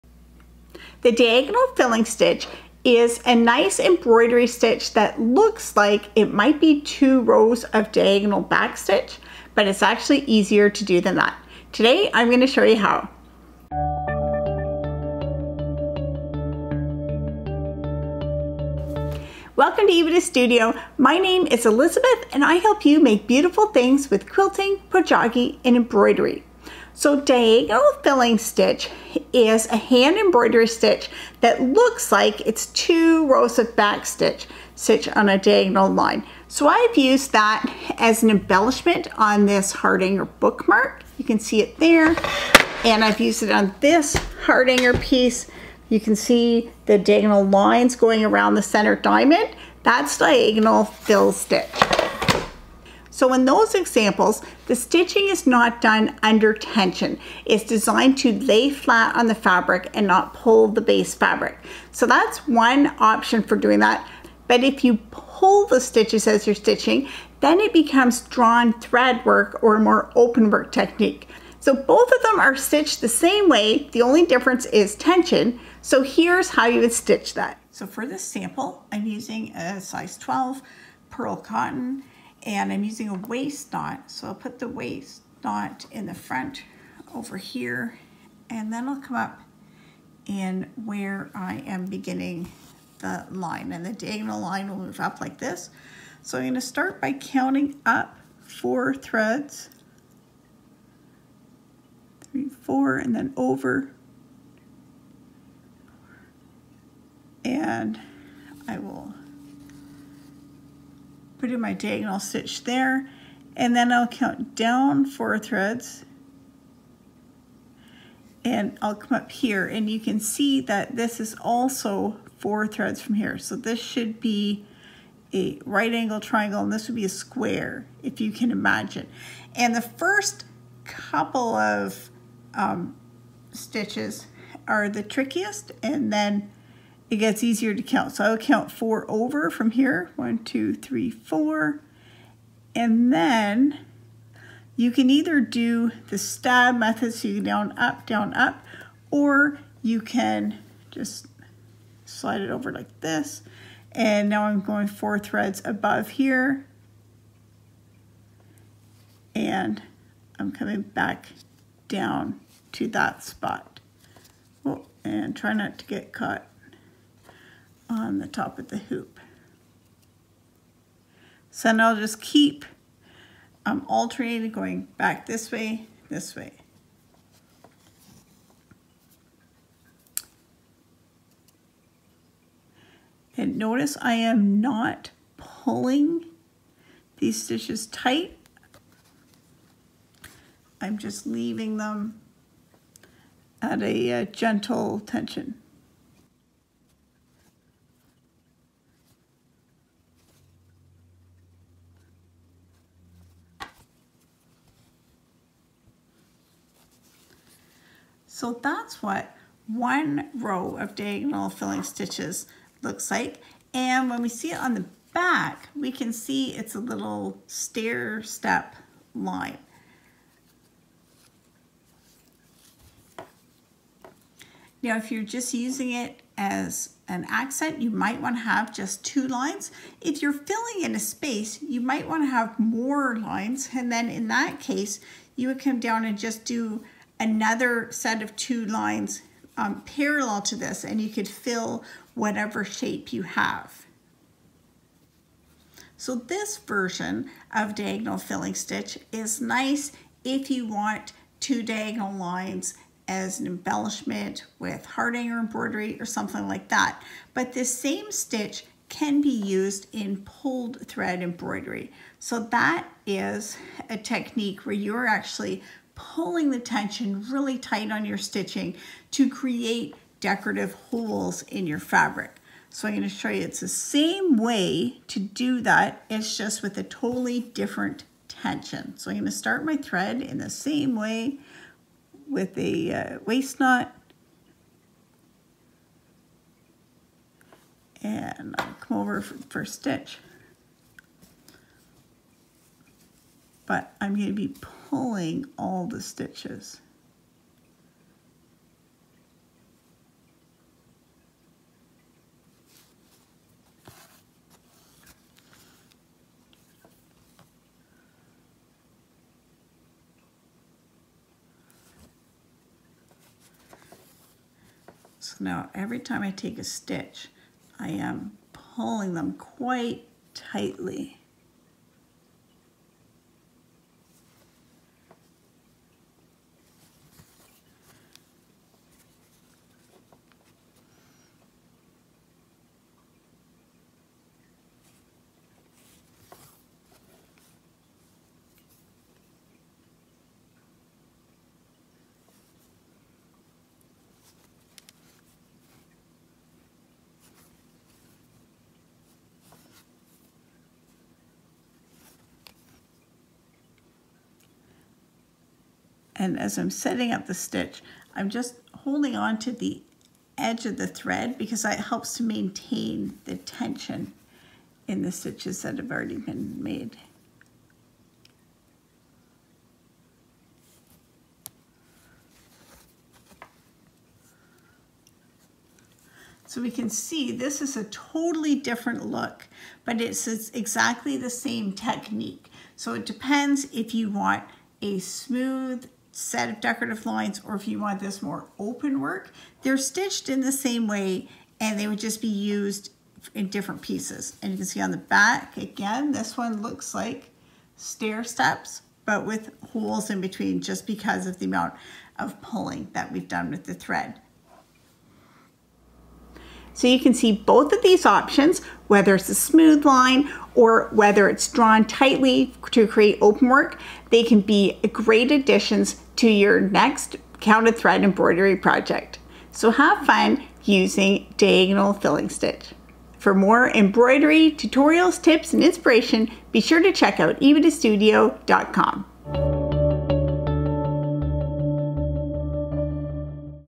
How to embroider Diagonal Filling Stitch
Diagonal Filling Stitch is a hand embroidery stitch that looks like two parallel lines of back stitch, but it's worked in a totally different way.

It is a great embellishment for a counted thread embroidery piece. If it is stitched under pressure, it is a beautiful pulled thread technique,